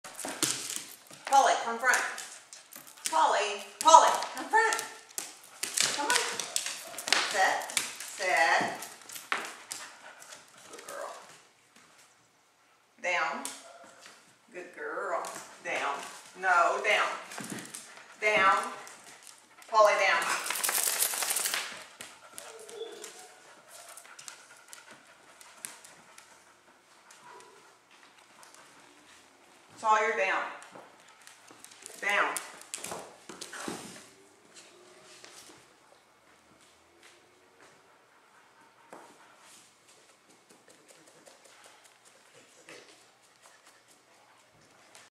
Polly in Doggy Boot Camp!
Polly sit, down, and come front! We are located in St. Louis Missouri.